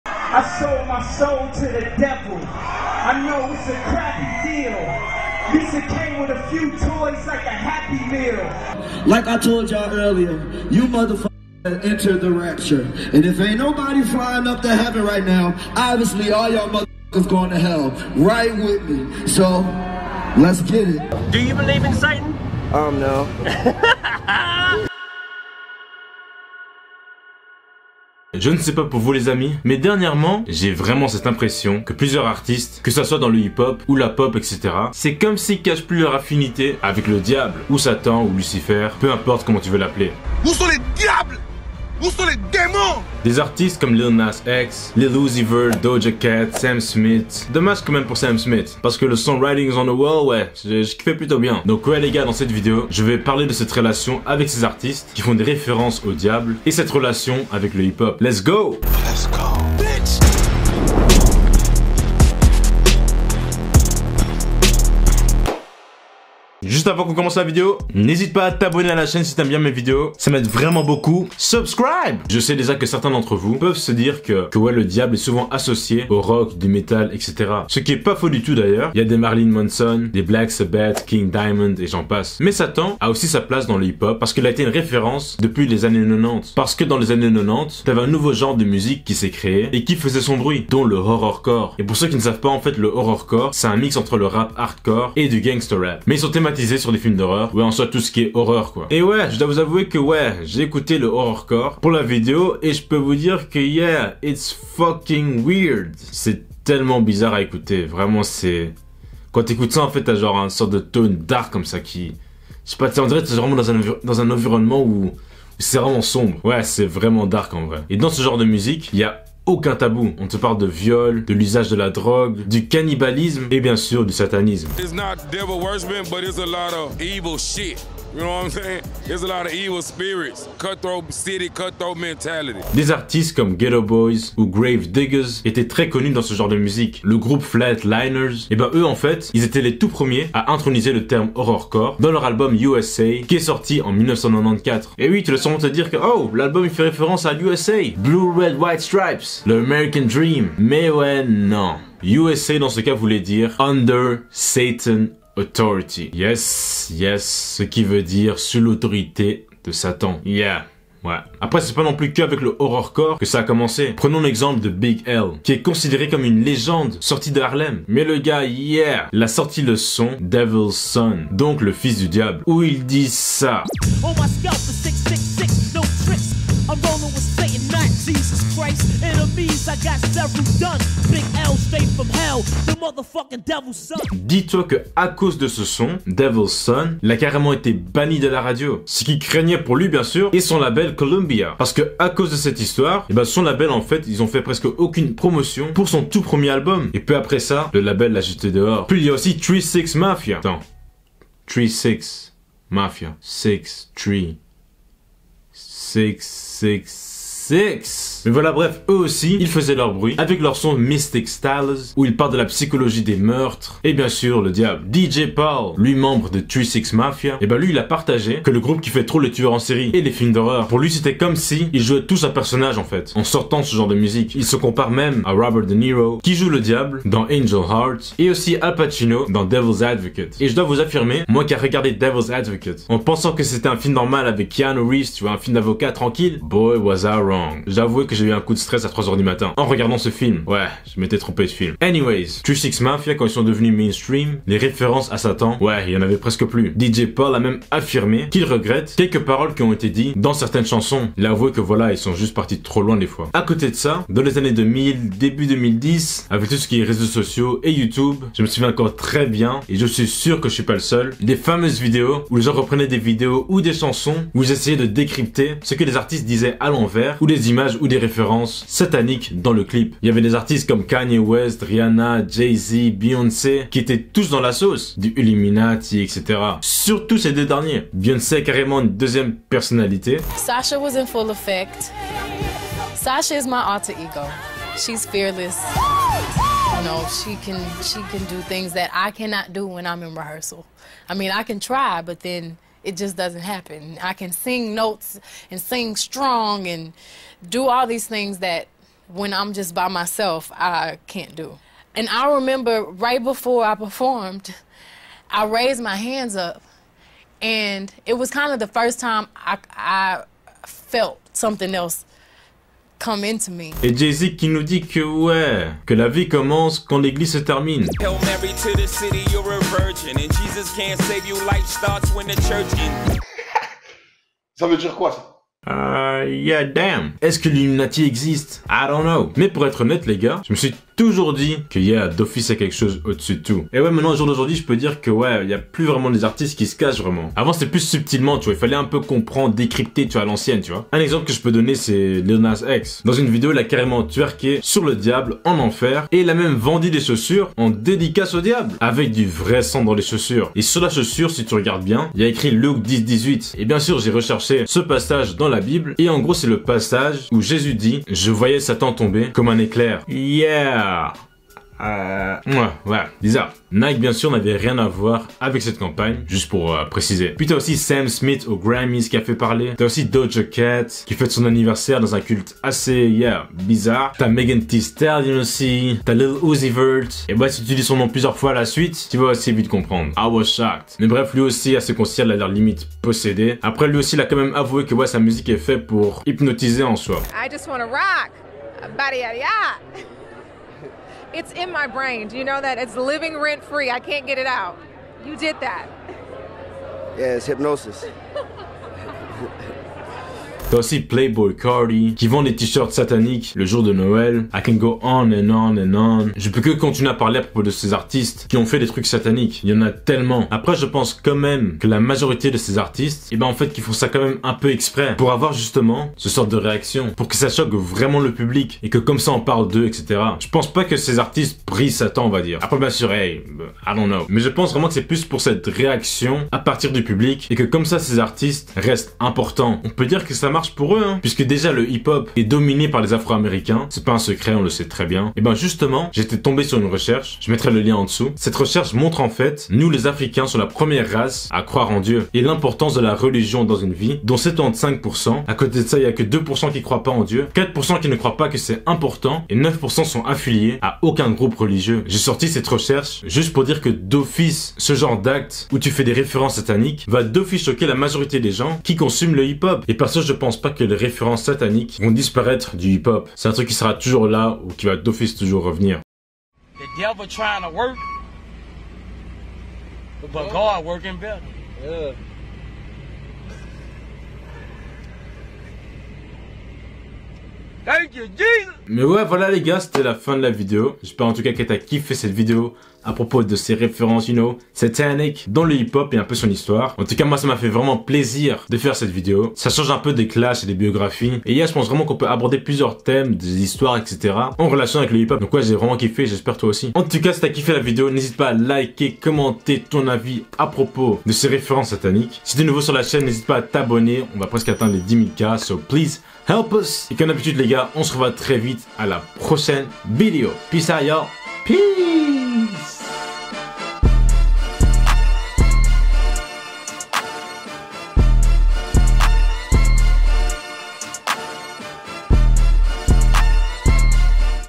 Le Satanisme Dans Le Hip-Hop
Dans cette vidéo on touche sur l'histoire du Satanisme/Horrorcore dans le monde du hiphop!
Pourquoi? Les Références? Quel impact? On vas parler de tout ça !
J'ESPÈRE QUE CETTE VIDÉO T’A PLU !
REJOINS MOI DANS CETTE JOURNÉE A 10K ABONNÉS